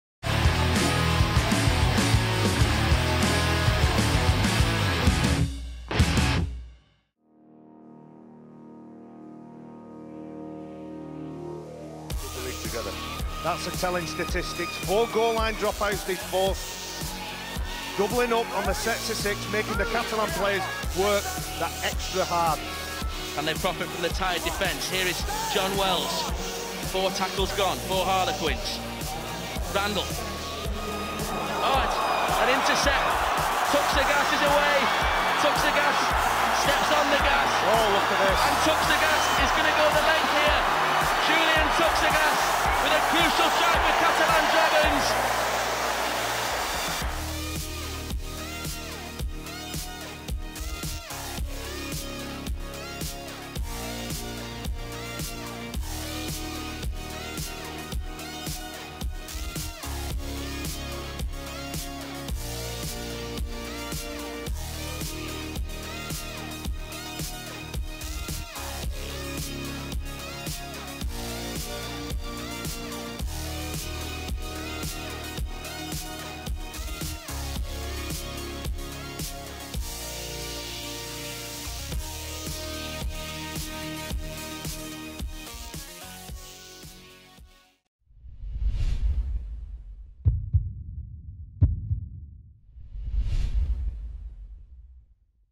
Magic WeekEnd
L'un des évènements marquants lors des Magic WeekEnd des Dragons, l'essai de Julien Touxagas en 2008 à la 70ème minute. Il permit aux Dragons de revenir au score puis de s'imposer à l'issue d'un match très disputé.